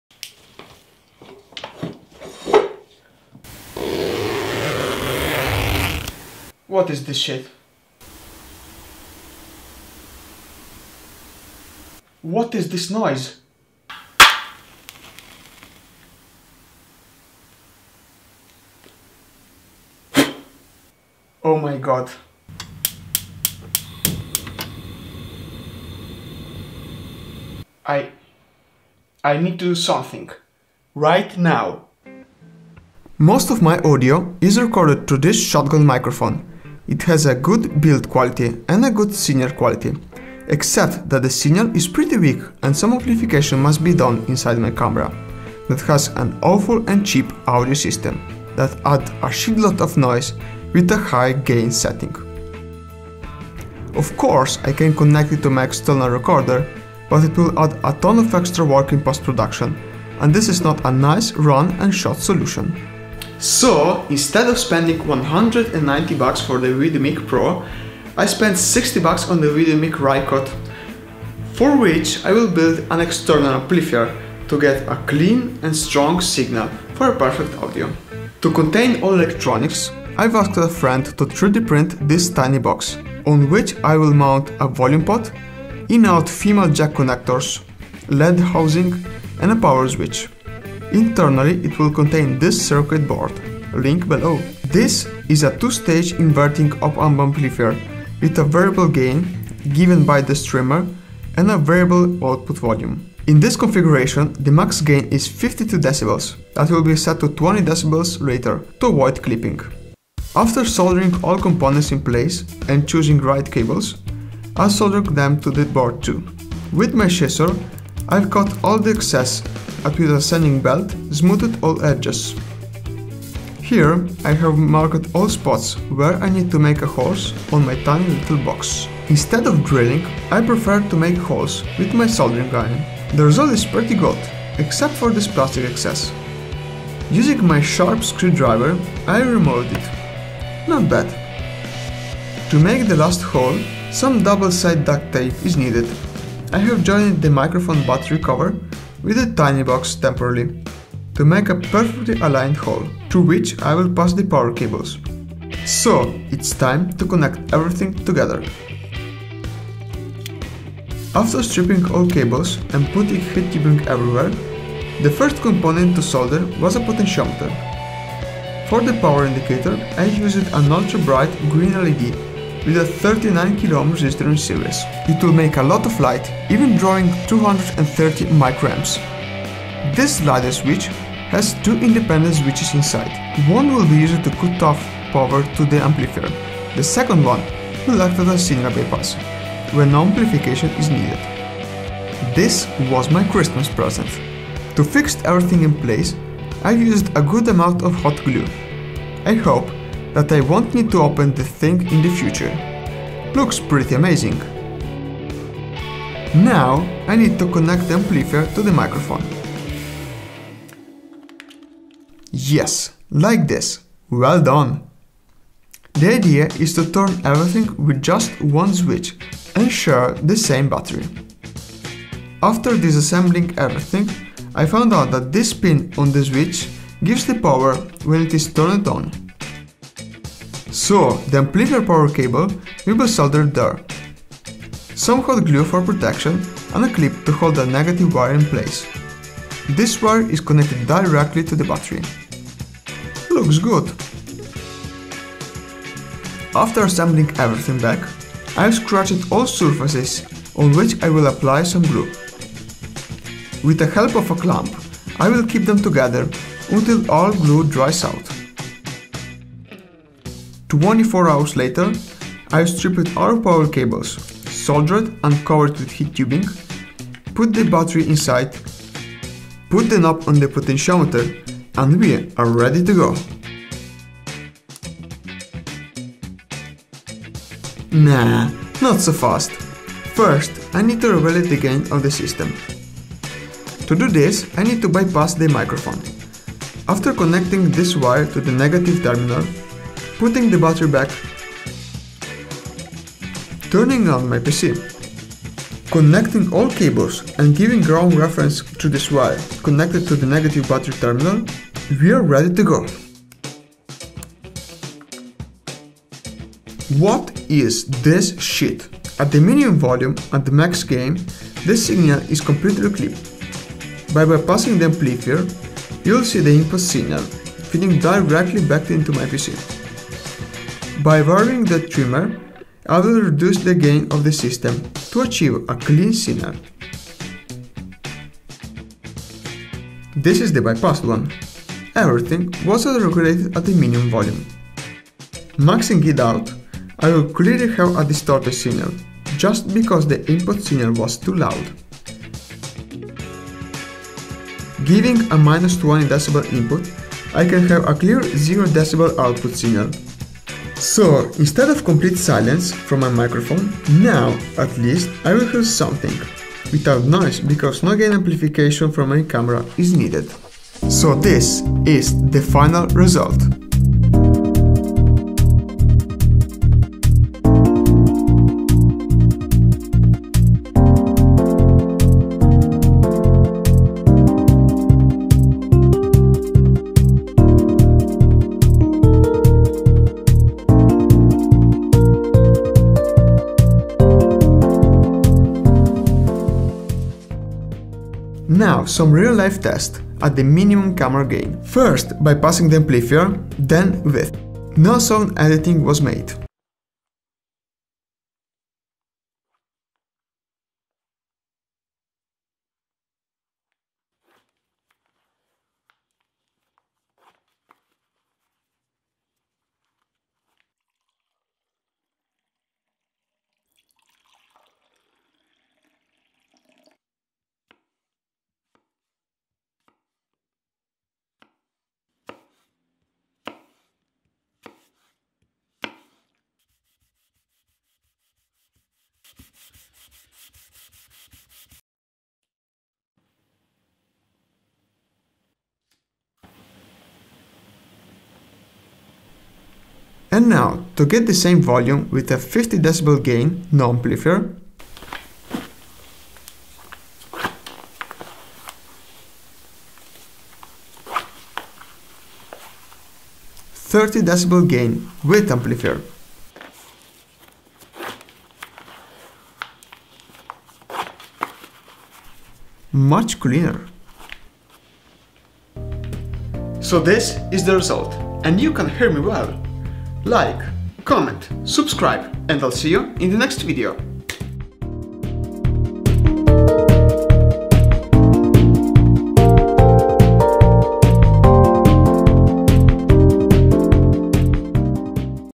DIY Microphone Amplifier for Shotgun Microphones
Back with a new audio preamplifier, because the last one was stolen together with all my gears :(

One of the biggest problem that i encountered during video production is bad audio. Audio makes 50% of the video. When a video have a bad audio, even with a greath content, it is very difficult to watch. I have a shotgun mic that works pretty well except for the signal intensity. It is pretty weak and my camera doesn't offer a good audio preamplifier. So i decided to make my own, external and directly connected to the microphone. The idea is to get a 20dB boost, with a variable volume, sharing the same battery and controlling everything with just one switch.